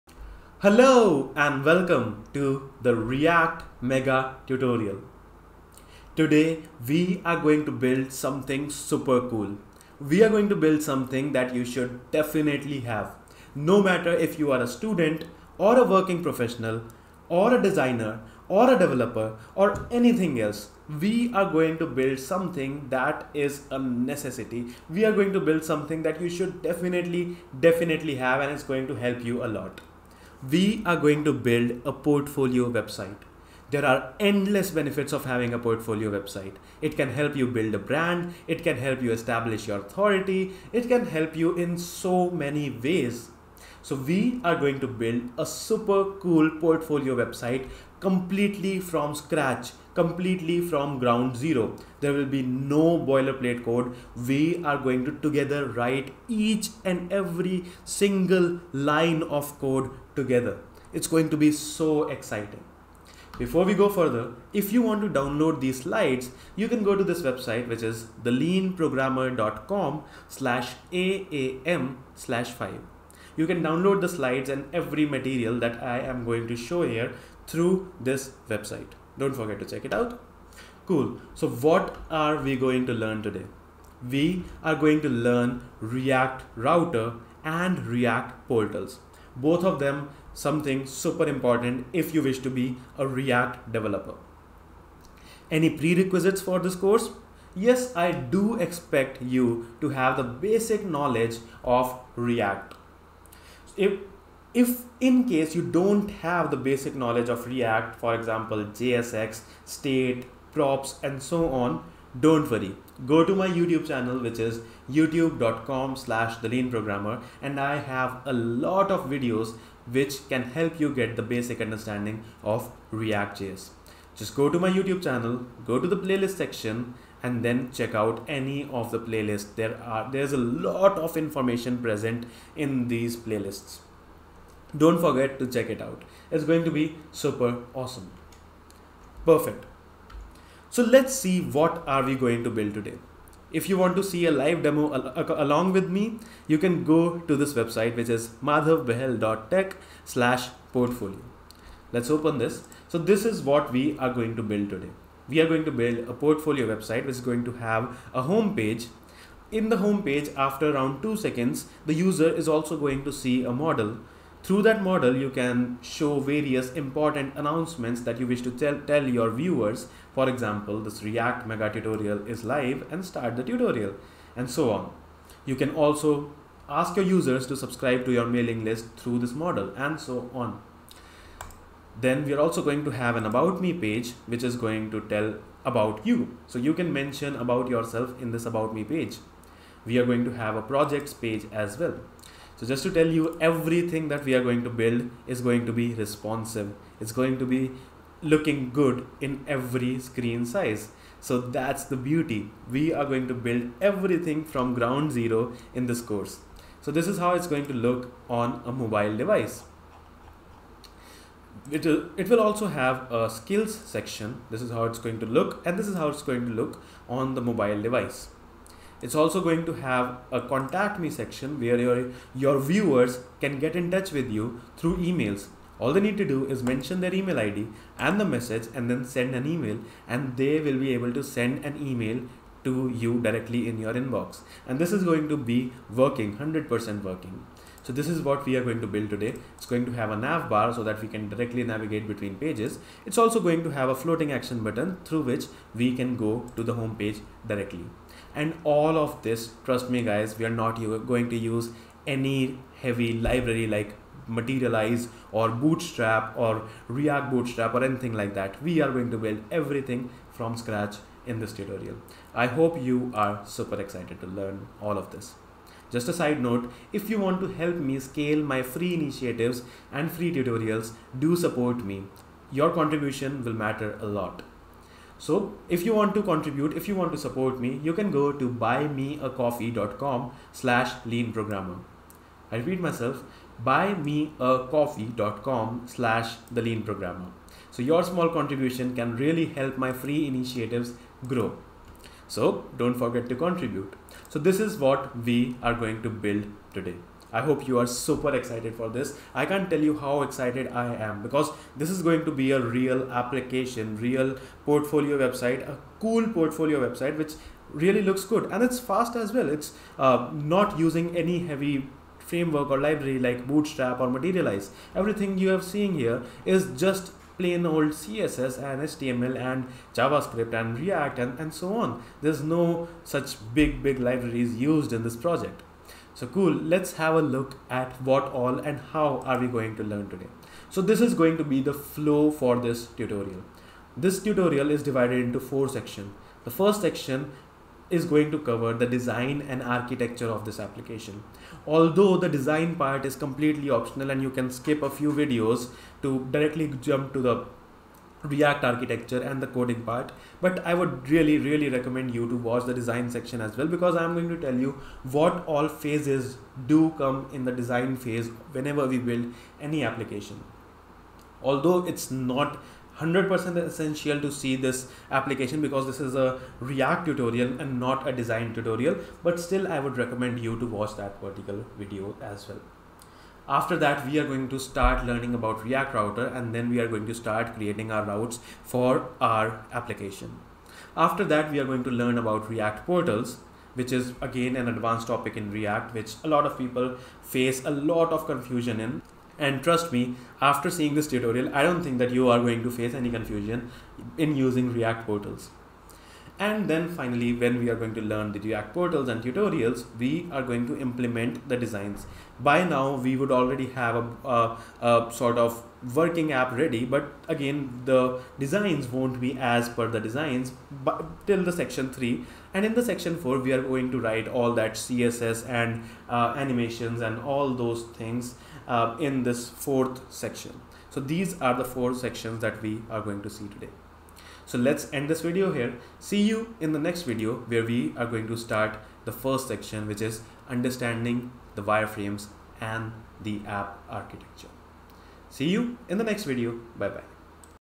React Mega Tutorial - Project 4, Part 1 | The Initial App Introduction
Welcome to the 1st part of the 5th AAM Project!

If you like my efforts, please contribute to my free initiatives

In this series, we are going to build a portfolio website completely from scratch,

In this video, we are going to introduce the various sections/phases of building this app.

Hope you enjoy the series ✨

Do post your progress on LinkedIn/Twitter and tag me 😁

Link to the GitHub profile where I will be pushing all my code